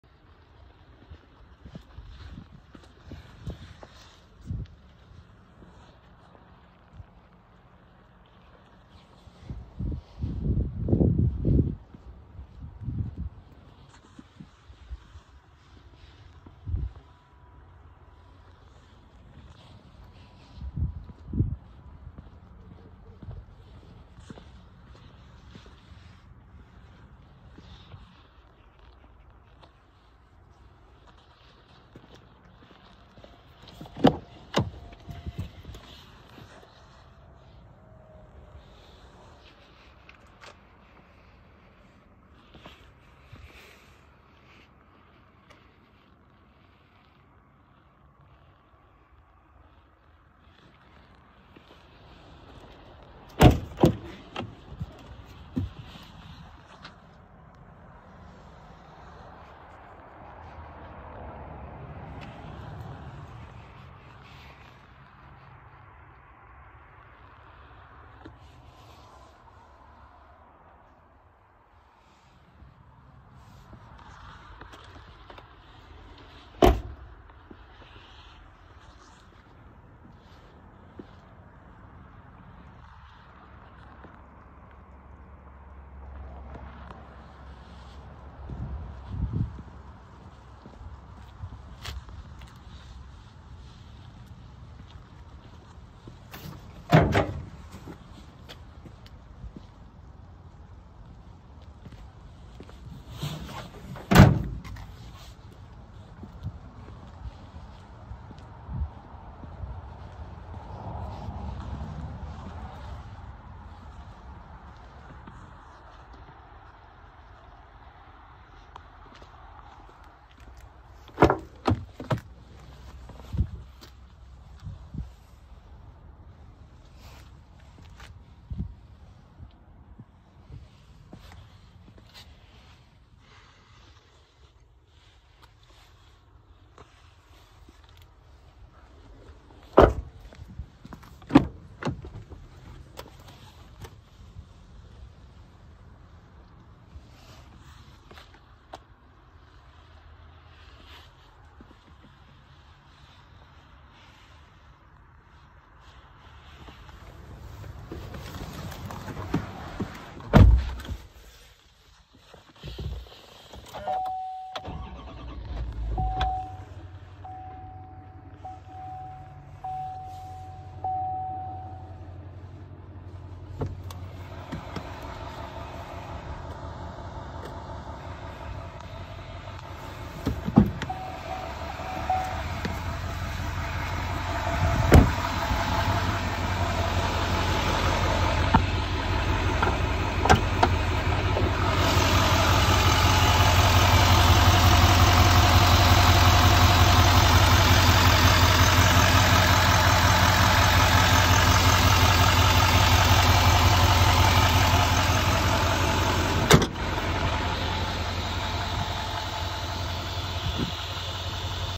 2022 Chevrolet Silverado K2500 HD LT STK#9669 // Replica Auto Sales
*DIESEL*
STK # 9669
KEY # 72
2022 Chevrolet Silverado K2500 HD LT
6.6L Duramax Diesel V8 Auto 4x4
43,717 Miles
Good Airbags!
Light front end damage
Runs and Drives on lot
$38,995